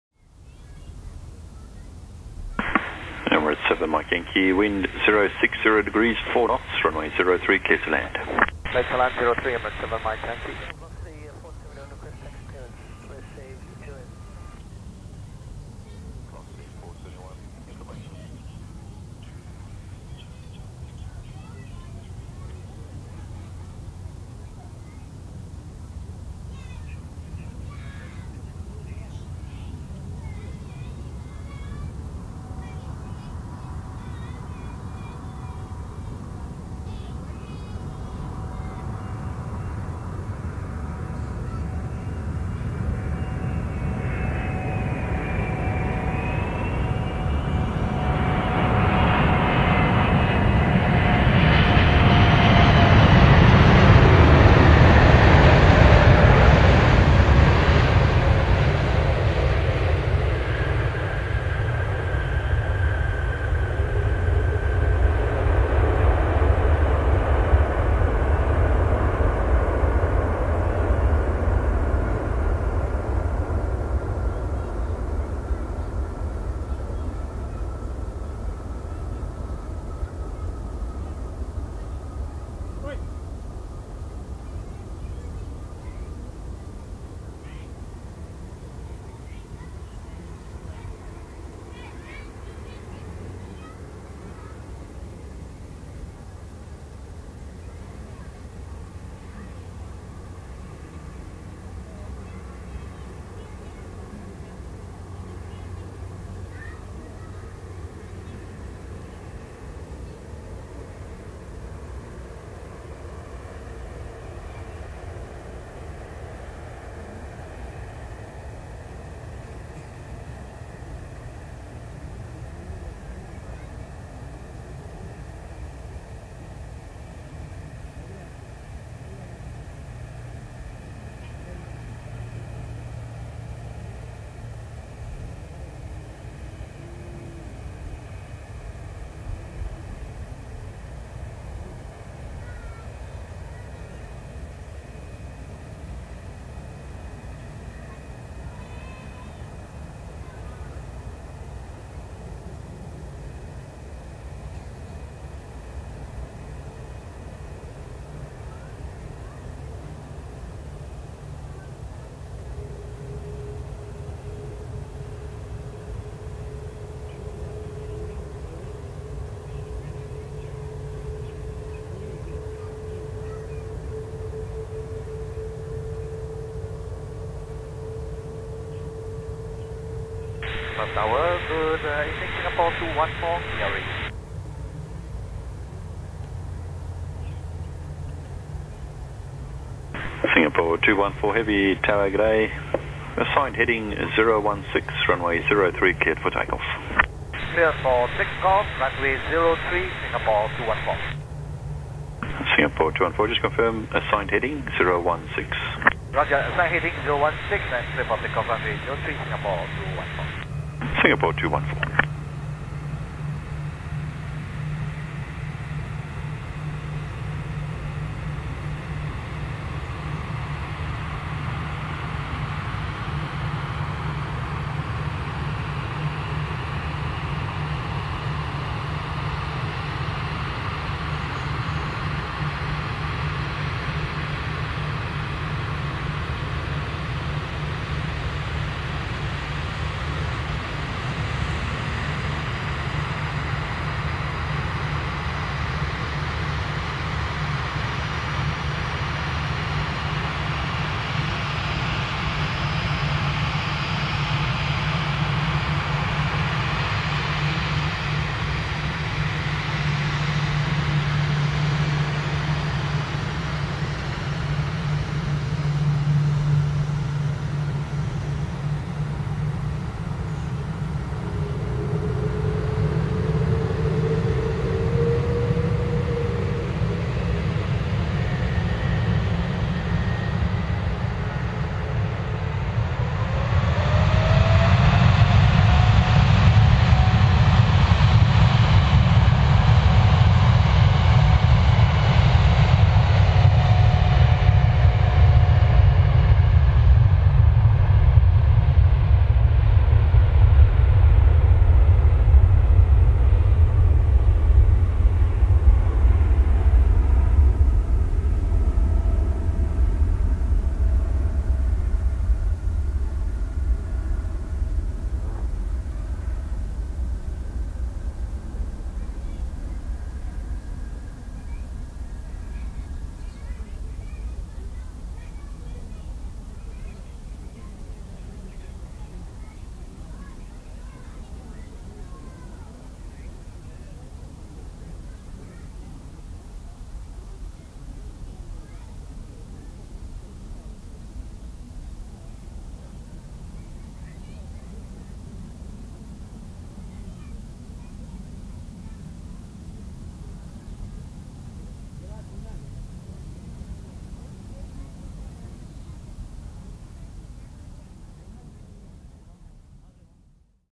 Emirates (A6-EOE) arrives and Singapore Airlines (9V-SCO) departs RW03 at Perth Airport.
In order of appearance:
Emirates (A6-EOE) [Rugby World Cup France 2023] Airbus A380-861
Singapore Airlines (9V-SCO) Boeing 787-10
on RW03 at Perth Airport.